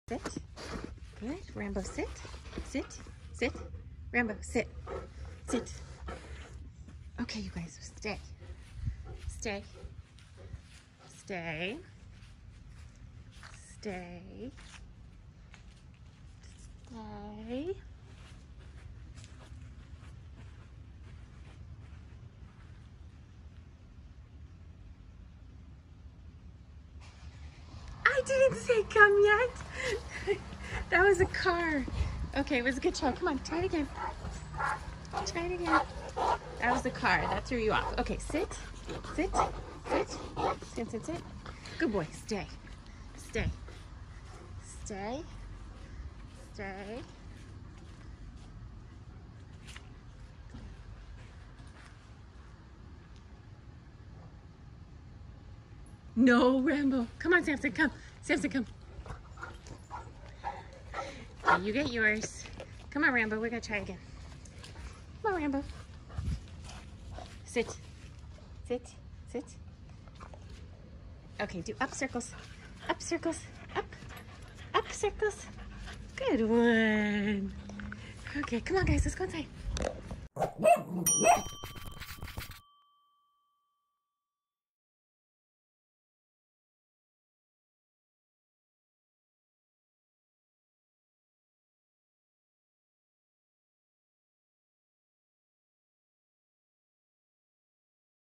Newfie and Cavalier have cutest training session
Rambo the King Charles Cavalier and Samson the Newfoundland are in the midst of a serious training session with their mom. Today on the agenda is the skill “sit and stay”. Samson is a pro at this one, but Rambo has a tendency to jump the gun a bit. Mom will have to give in and find another way to reward Rambo with his coveted treat!